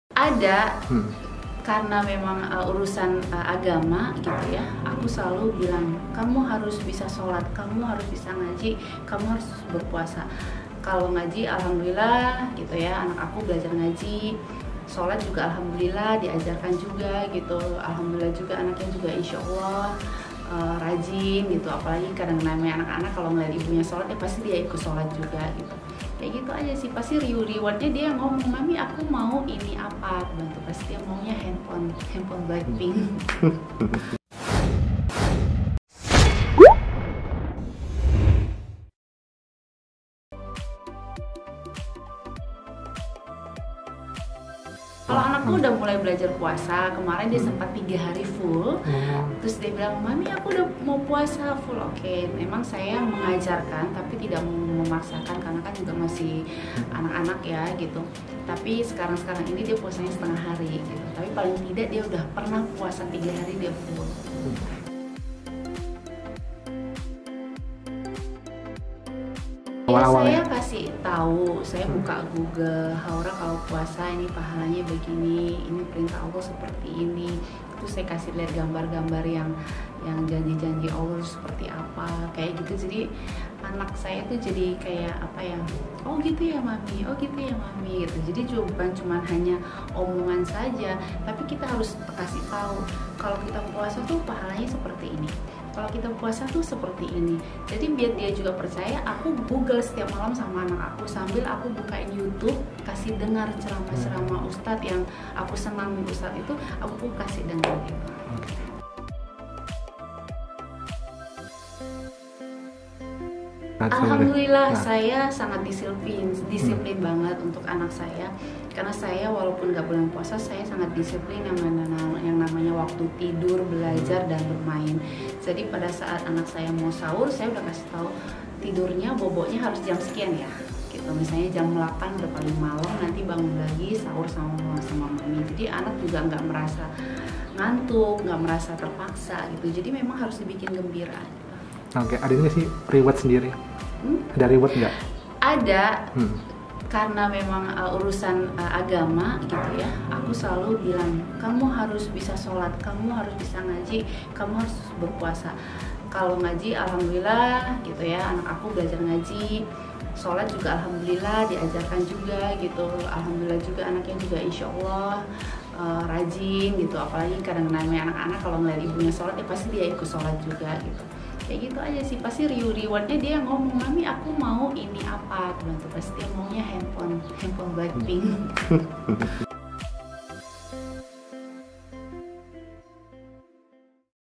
Angel Lelga Ajari Anak Berpuasa
Ramadhan tahun ini mungkin sangat berarti bagi Angel Lelga. Bagaimana tidak, pada puasa Ramadhan tahun ini Angel harus lebih bersabar karena dirinya harus membimbing anak-anaknya dalam menjalankan ibadah puasa.
Seperti apa serunya seorang Angel Lelga dalam mengajari anaknya berpuasa?